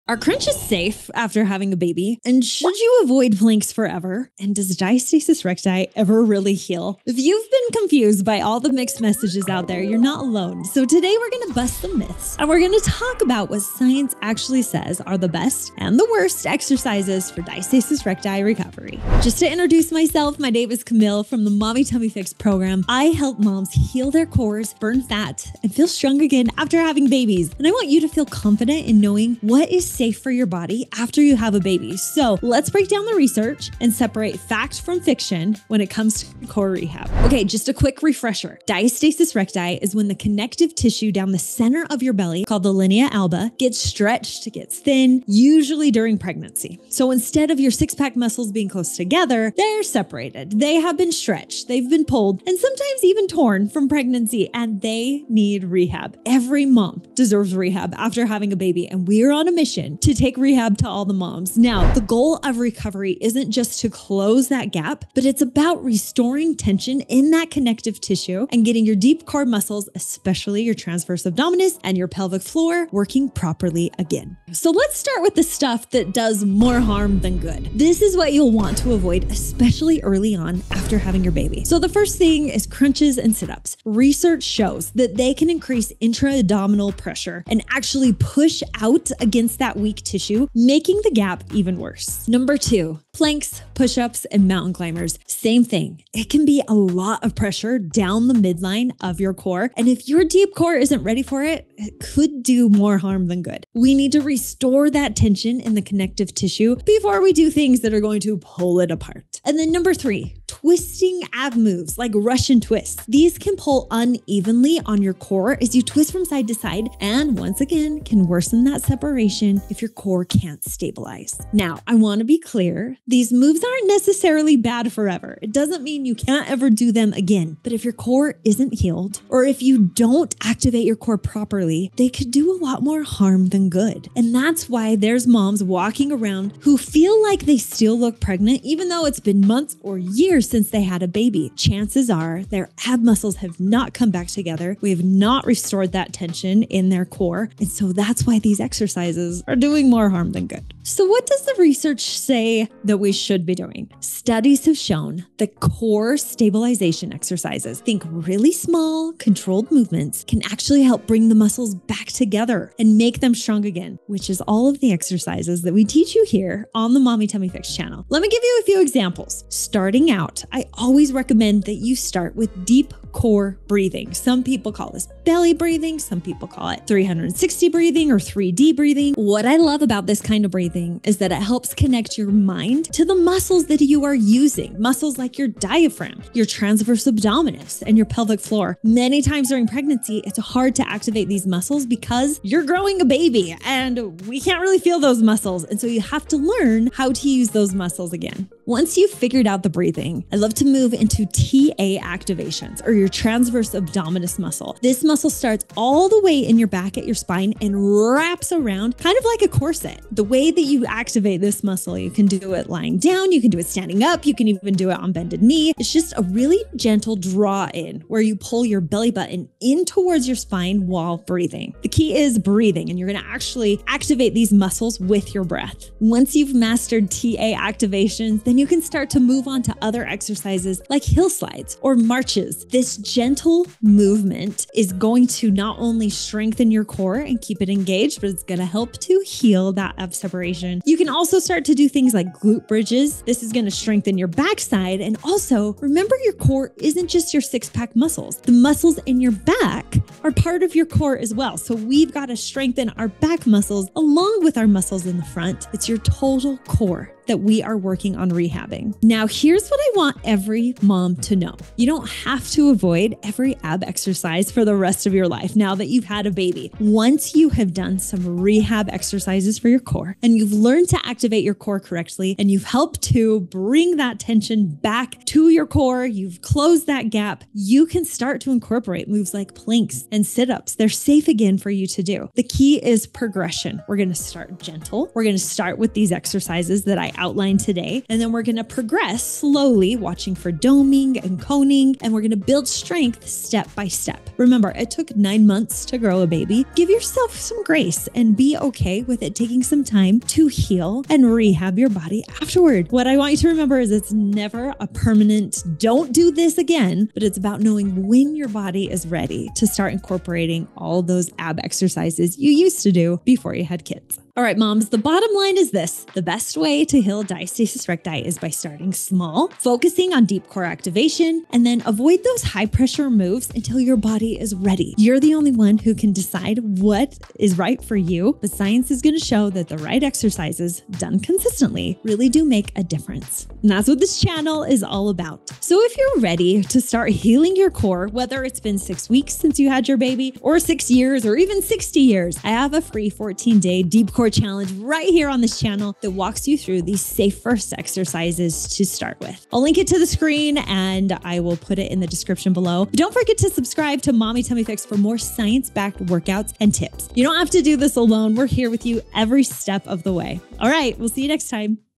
BEST (and WORST) Exercises for DIASTASIS RECTI Recovery (According to Science)
If you’ve ever wondered which core exercises actually help heal diastasis recti — and which ones might make it worse — this video breaks it all down for you.

In this video, we’ll dive into the latest research (2025) on core rehab and share what science says about:
✅ The best exercises for diastasis recti recovery (and why they work)
🚫 The common moves that can actually slow your healing
✅ How to safely strengthen your deep core and reduce intra-abdominal pressure
✅ Simple cues to help you engage your transverse abdominis the right way

I know firsthand how confusing postpartum fitness can be — so I’m here to simplify the science and help you feel confident healing your core in a gentle, effective way.

READY TO START HEALING YOUR CORE?

📗 Camille’s favorite books

AMAZON STORE

VIDEO REFERENCE POINTS: